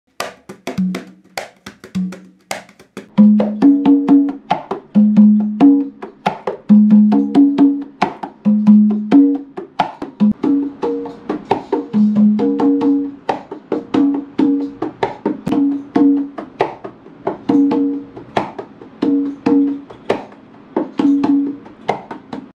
CONGA GROOVE VARIATIONS
Notice the subtle differences? Hear this and a lot more on our first upcoming percussion loop pack featuring Jacques Dufourt!